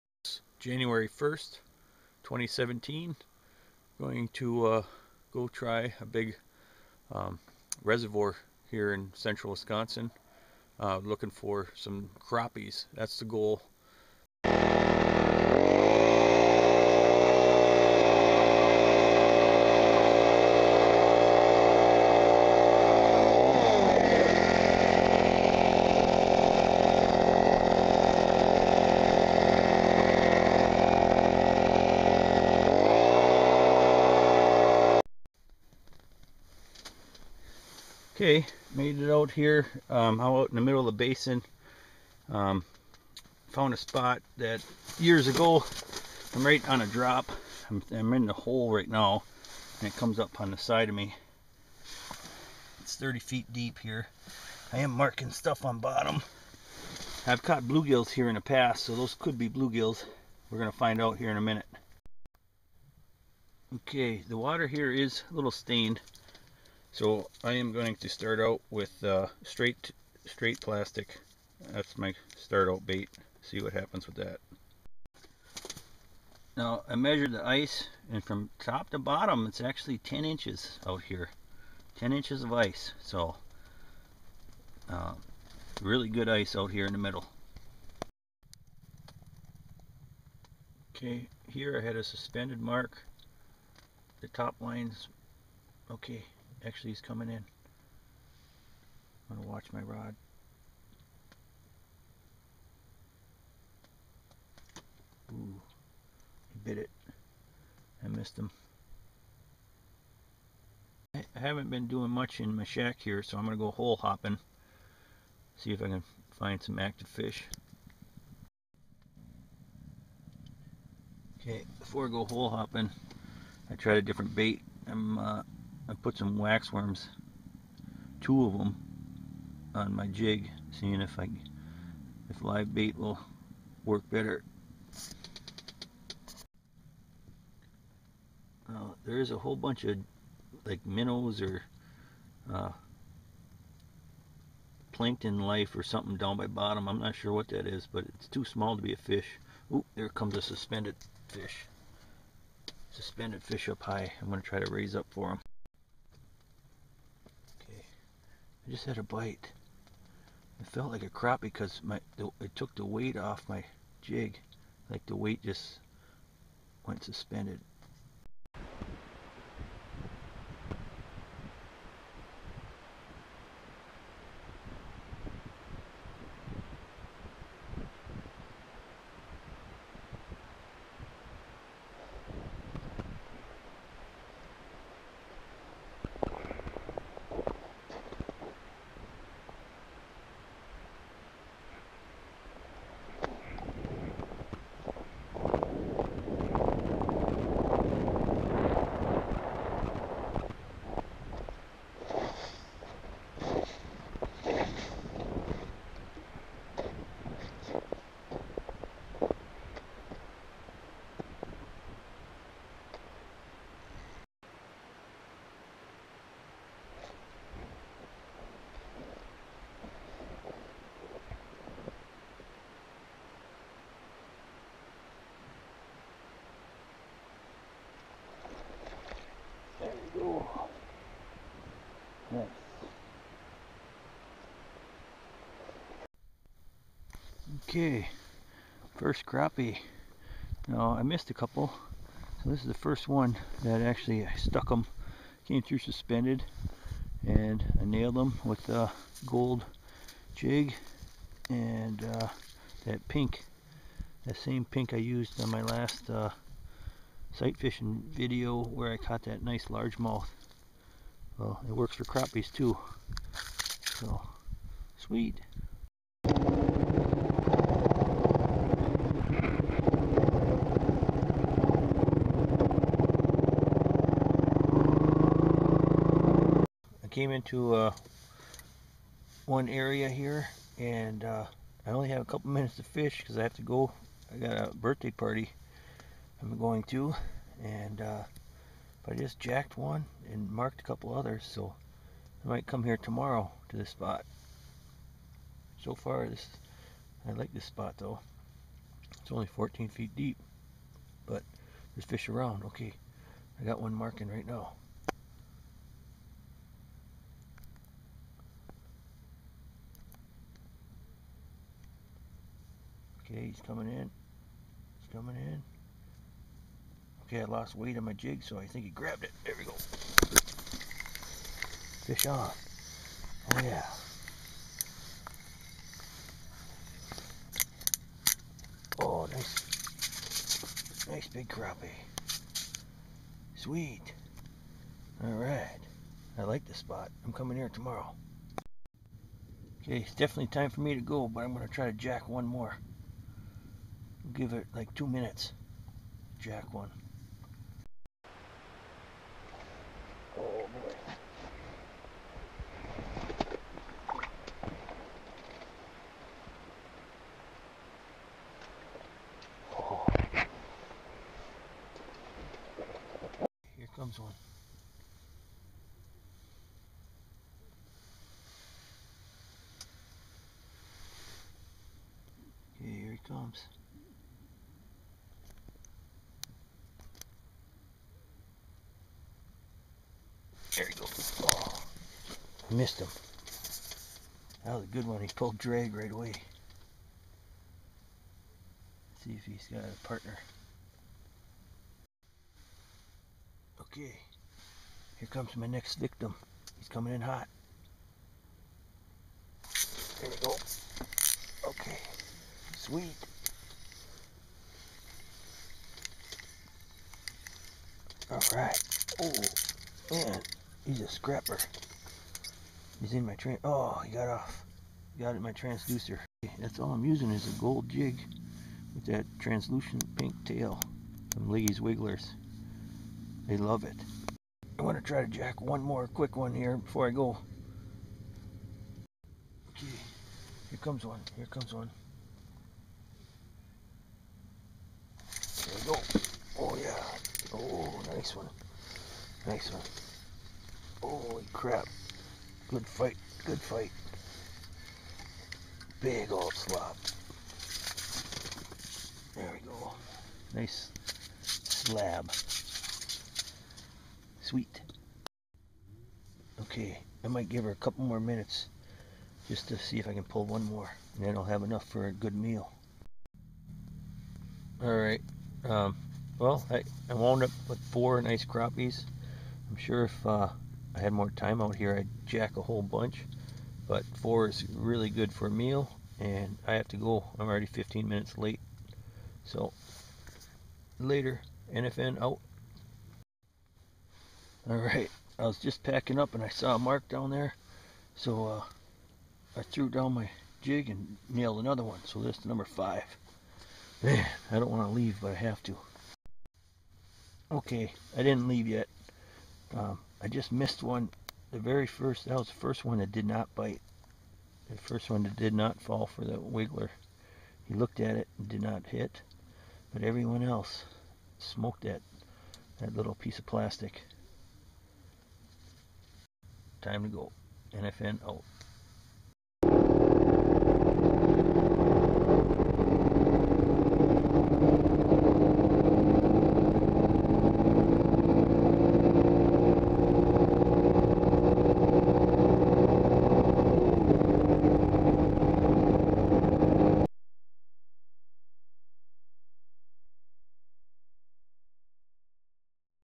NFN Icehole Adventure #4 New Years Day Crappie Action 2017 - Ice Fishing Icefishing
Scouting for some Crappie on a central Wisconsin reservoir January 1st  2017